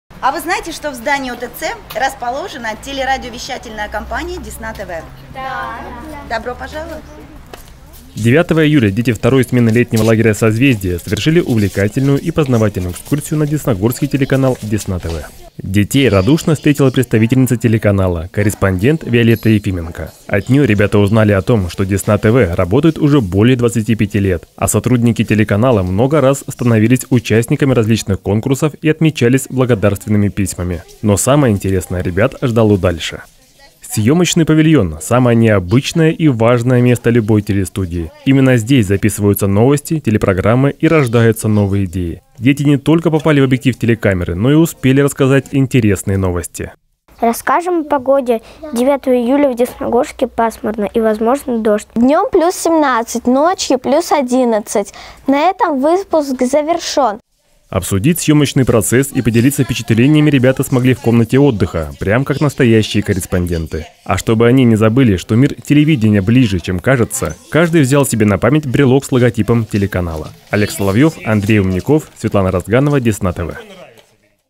Десна-ТВ: Как делается телевидение: экскурсия на телеканал «Десна-ТВ»
Эфир телеканала, съемка телепередач и сюжетов – это результат большой работы, которая остается за кадром. Что происходит за кулисами съемочного процесса теперь знают дети из летнего лагеря «Созвездие».